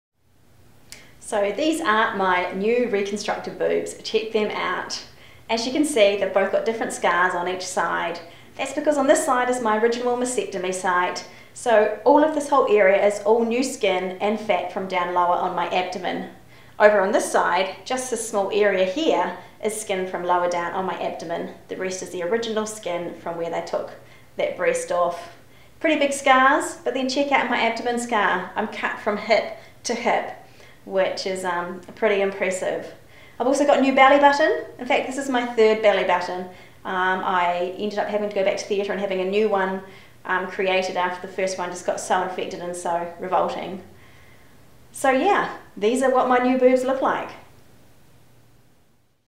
My Breast Reconstruction - Sarah's Breast Cancer Journey
This is my breast cancer journey and I would like to show the results of my breast reconstruction surgery. It is 128 days after my reconstruction surgery was undertaken and nearly 3 years since my initial mastectomy surgery.

I am going to share my story from the beginning till the end as a series of blogs. I hope that by telling my story I will help other women understand what might lie ahead.
I'm not living for the what ifs, I'm living for the what is!